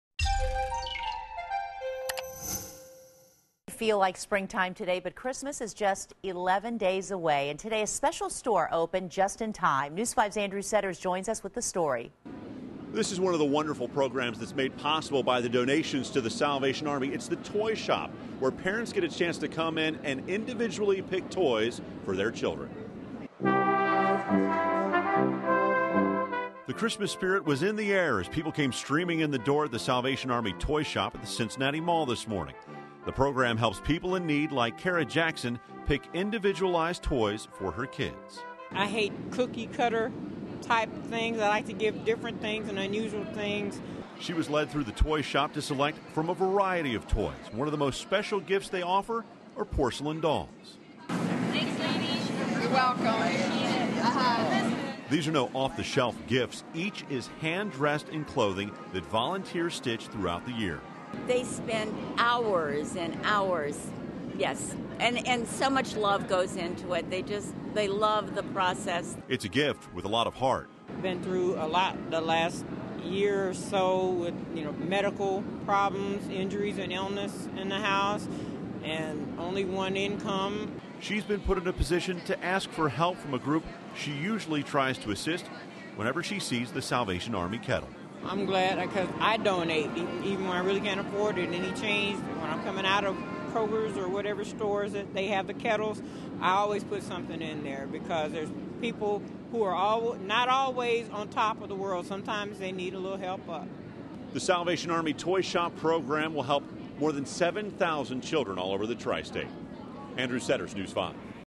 Salvation Army Toy Shop Helps Families In Need
A special store opens just in time to help some needy families provide their children with Christmas presents.